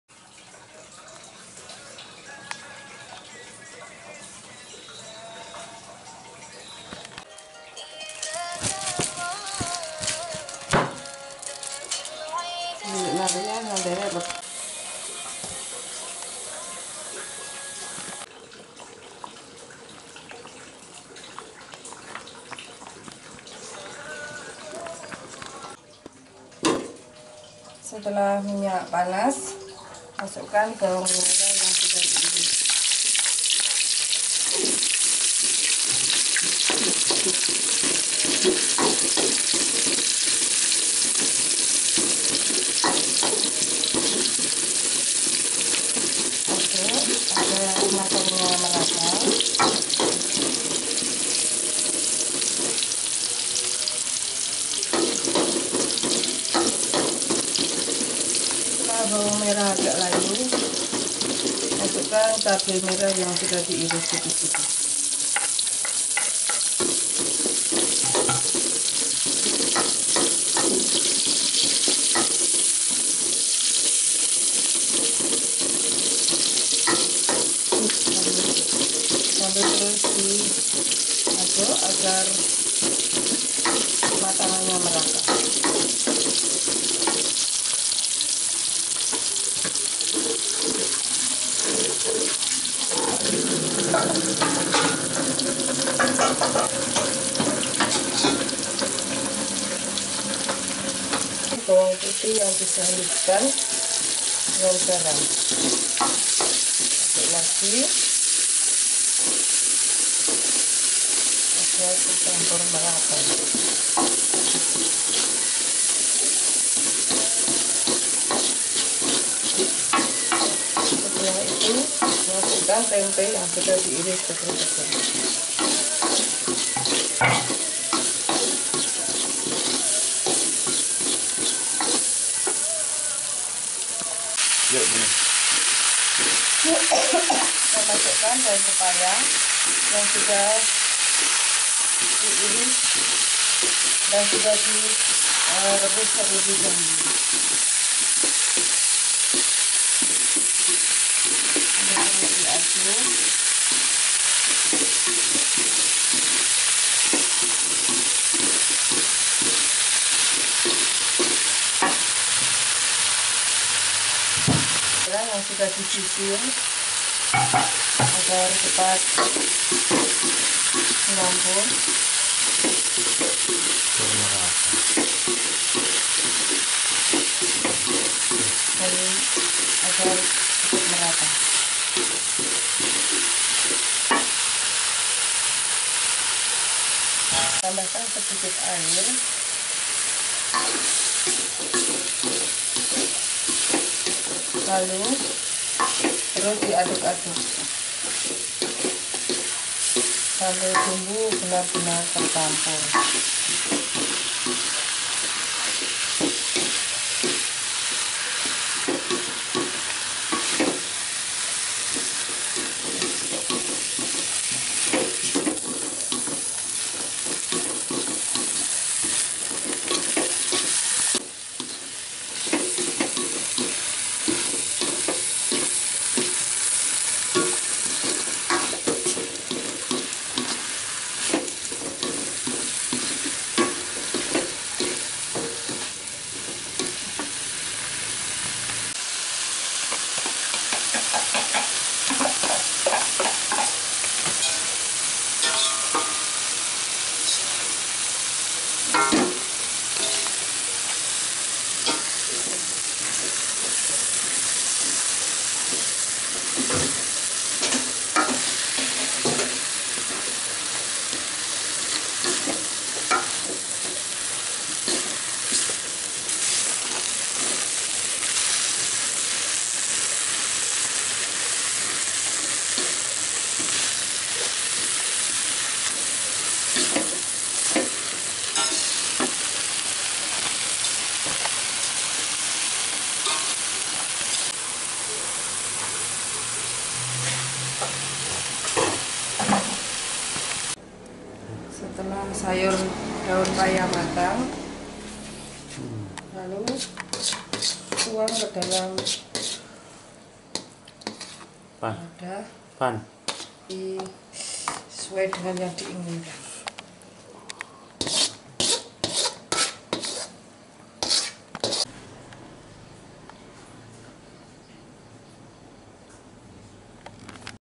Masak Oseng Daun Pepaya Tidak Pahit & Tetap Hijau
daun singkong
 bawang putih
 bawang merah
cabe rawit merah (diiris
buah cabe merah keriting
2 lembar daun salam

Lengkuas seruas jari
buah tomat
 minyak goreng untuk menumis
Secukupnya gula,garam, Masako
Secukupnya air